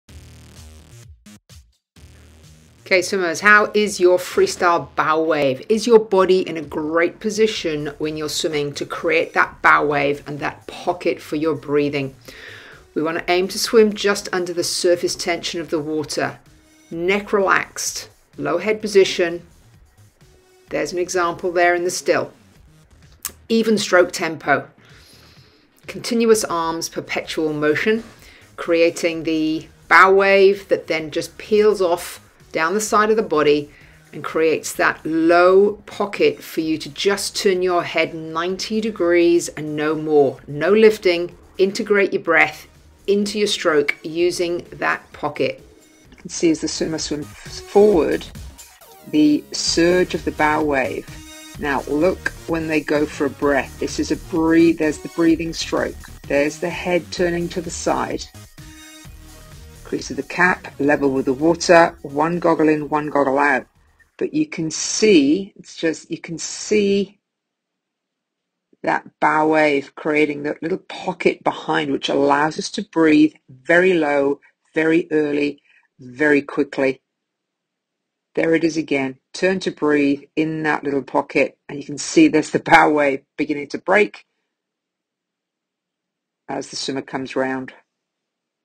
How is your Freestyle Bow Wave?
Does your stroke create a bow wave when moving through the water?
As a coach, one of the aspects of an improving stroke we observe - is the creation of a continuous bow wave.

The bow wave is also handy for breathing, as it allows the swimmer to find a 'low pocket' of air, to sip. It allows the swimmer to turn a mere 90 degrees or less. in order to integrate smooth breathing around the stroke cycle!

Creating a bow wave comes from:
Neutral spine line and relaxed neck position
Even stroke tempo
Perpetual motion of the stroking arm
Smooth transition of entry and catch components